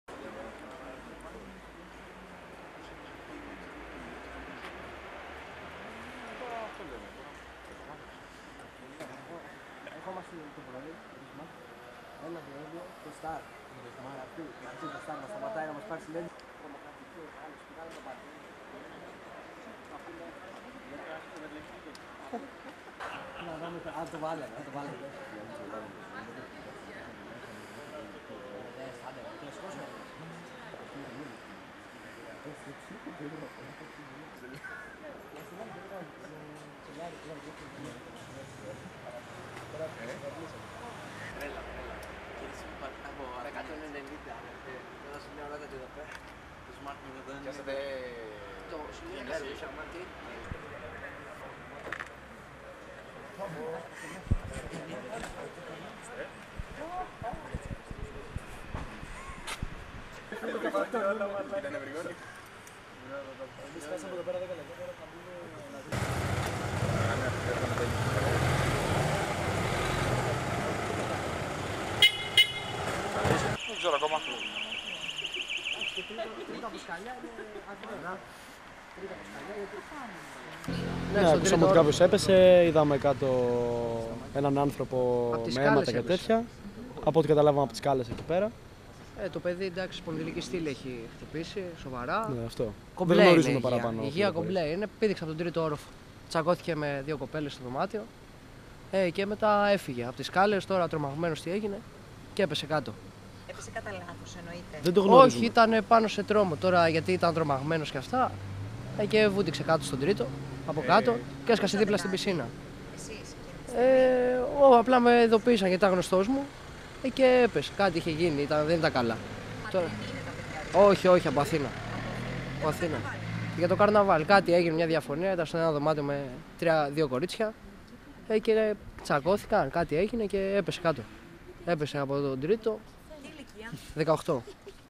Πάτρα | Πτώση 18χρονου από μπαλκόνι ξενοδοχείου
Αναστάτωση επικράτησε το βράδυ της Παρασκευής σε γνωστό παραλιακό ξενοδοχείο της Πάτρας, μετά την πτώση 18χρονου από μπαλκόνι. Ο νεαρός μεταφέρθηκε εσπευσμένα στο Πανεπιστημιακό νοσοκομείο στο Ριο, με την κατάσταση του να είναι κρίσιμη. Σύμφωνα με τις πληροφορίες, φέρεται πριν την πτώση προηγήθηκε διαπληκτισμός στο δωμάτιο που διέμενε.